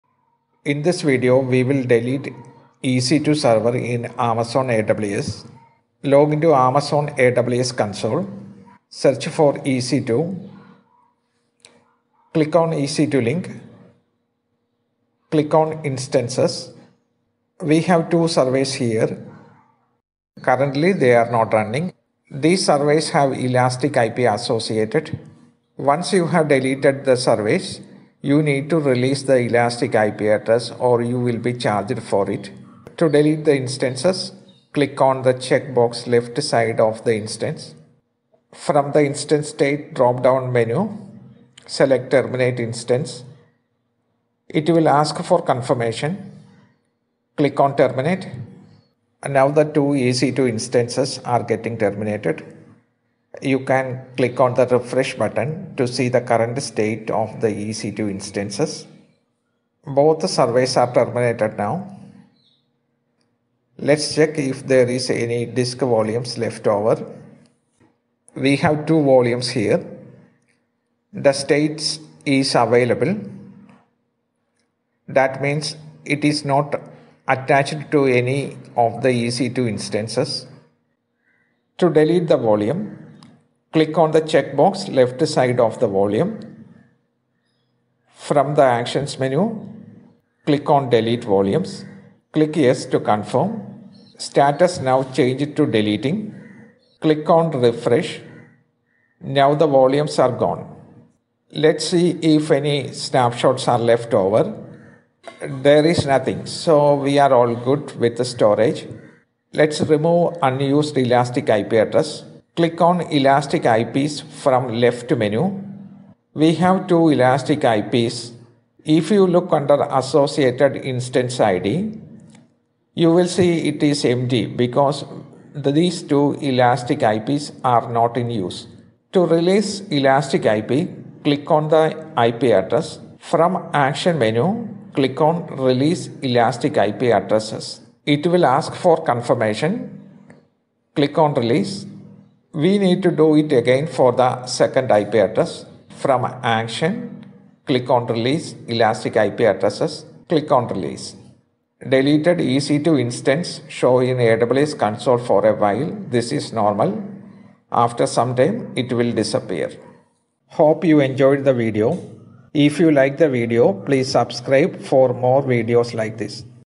How to delete Amazon EC2 instance
Terminate an Amazon AWS EC2 server.  Release Elastic IP address. Delete unused storage volumes.